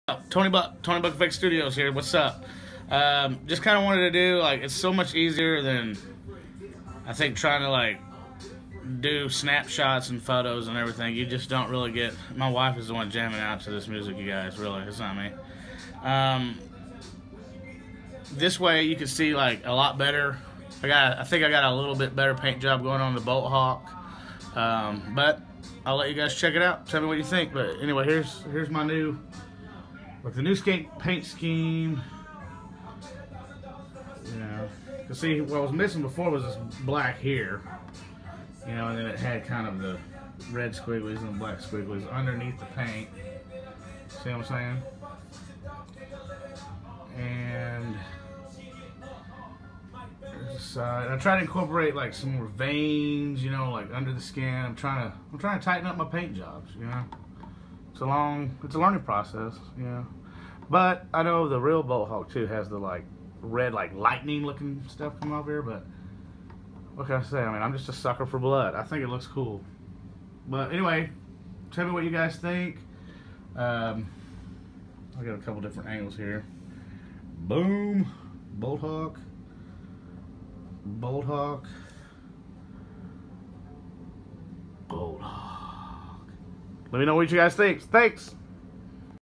Slipknot Bolthawk
Finishing up my own version of the Bolthawk!!